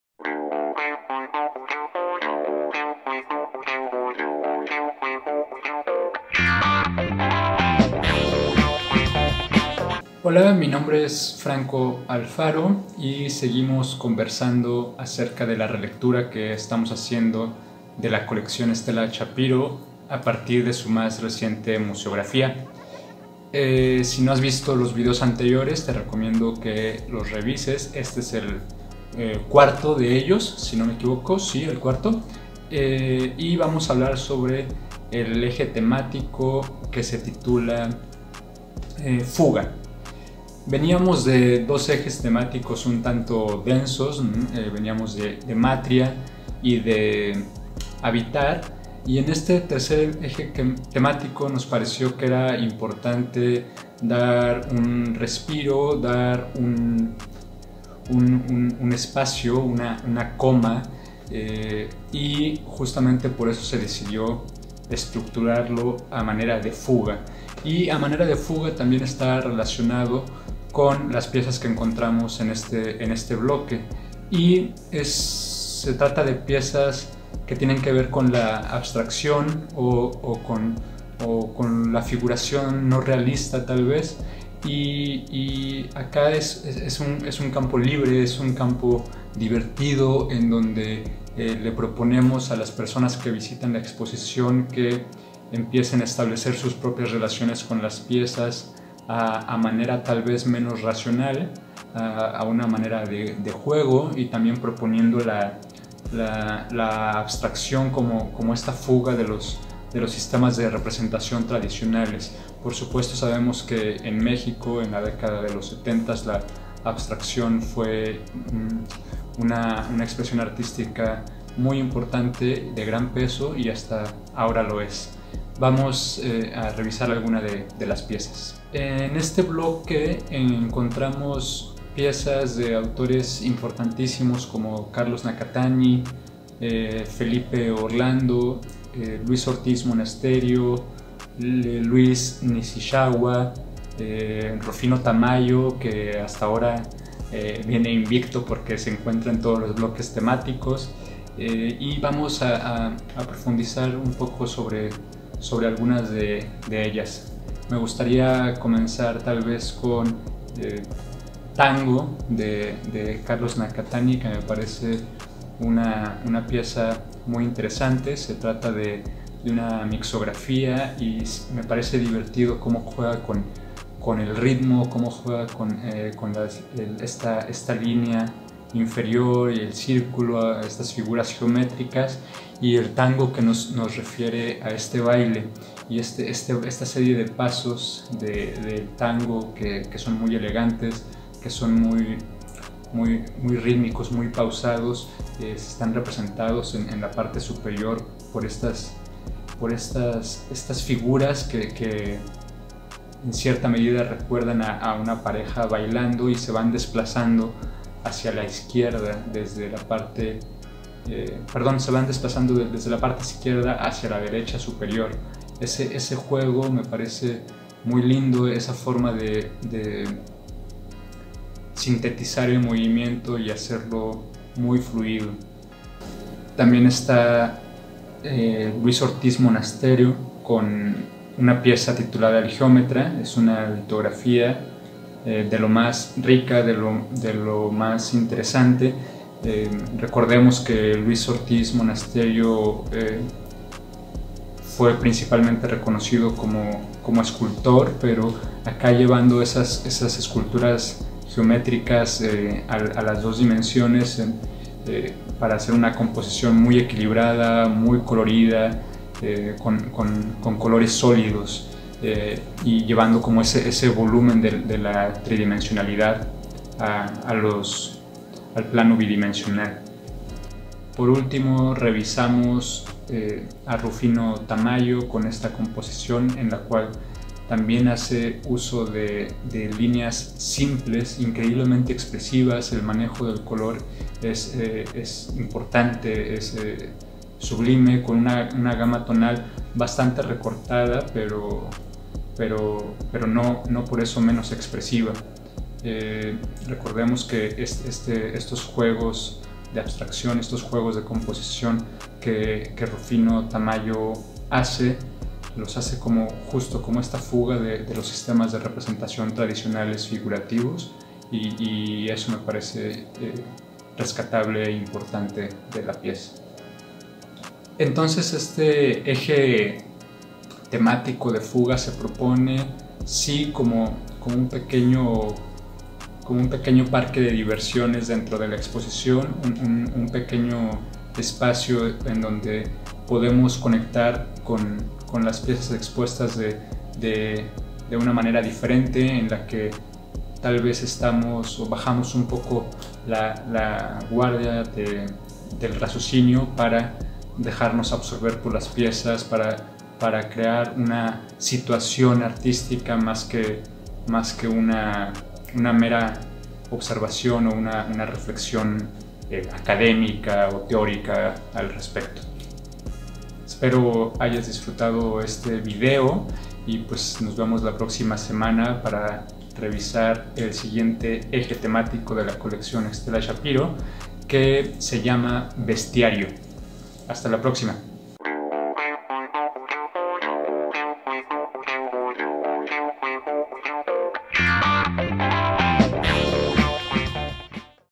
Episodio 4: Fuga - Releamos: Colección Estela Shapiro
Adéntrate a nuestra nueva sección "Releamos: Colección Estela Shapiro" de la mano de Franco Alfaro, asistente de galerías del Centro de Difusión Cultural Raúl Gamboa del IPBA y docente de fotografía del Instituto Potosino de Bellas Artes; quien nos comentará a detalle y en formato de serie, acerca de la emblemática colección "Estela Shapiro", que forma parte del acervo del Centro de Difusión Cultural Raúl Gamboa del IPBA.